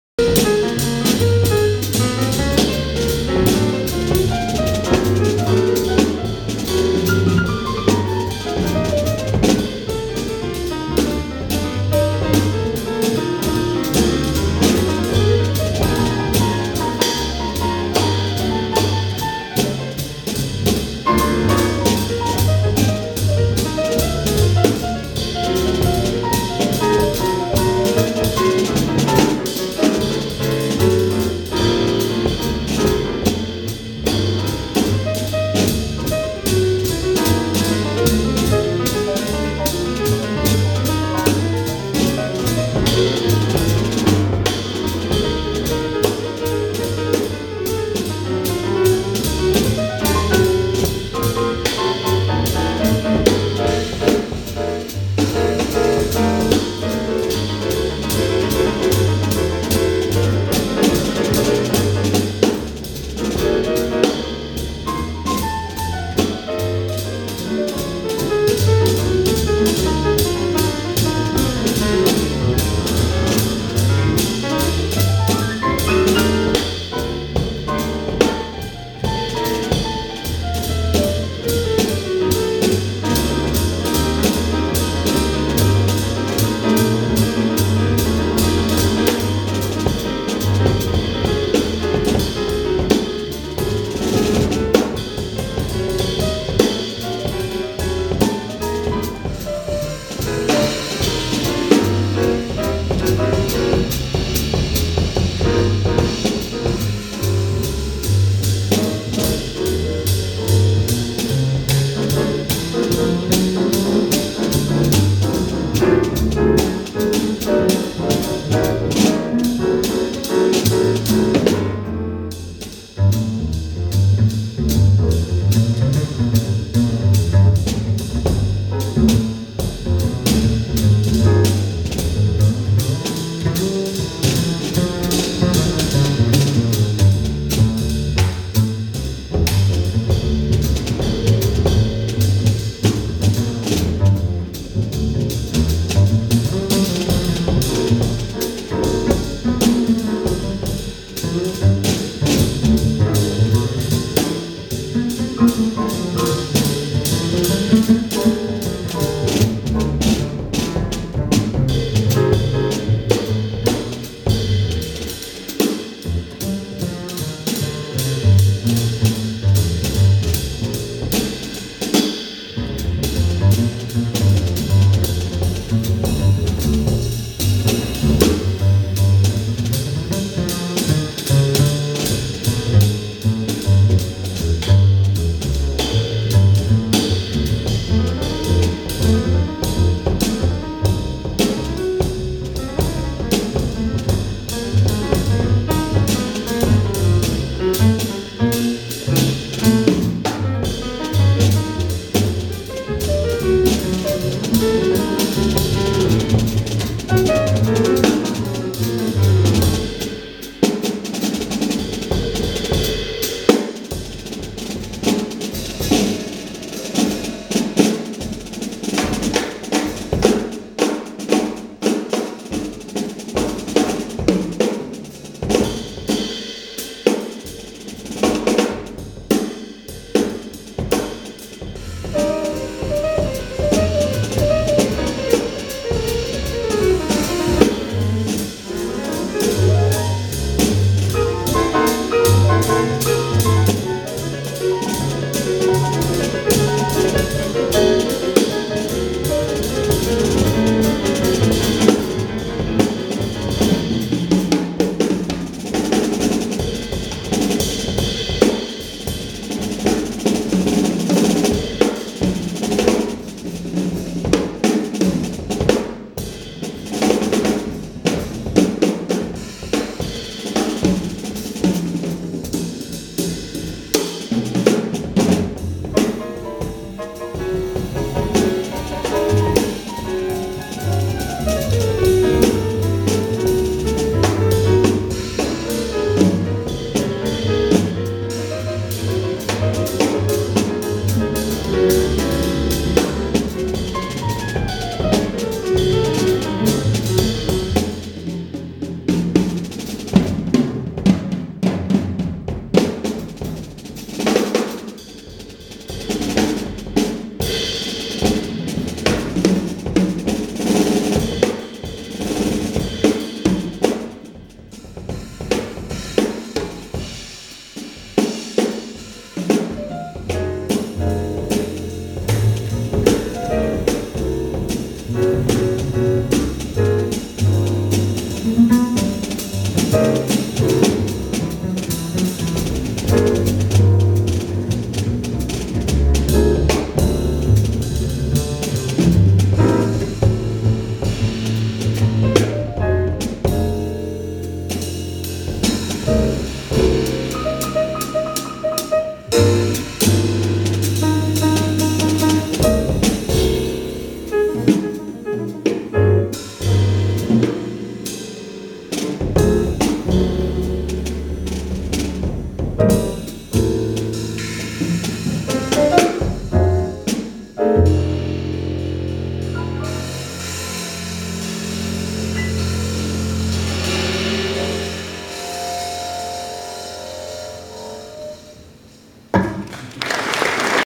John Taylor Trio - (1) - may2011
John Taylor (p) - Palle Danielsson (b) - Martin France (dr)
Excerpt from concert in Hamburg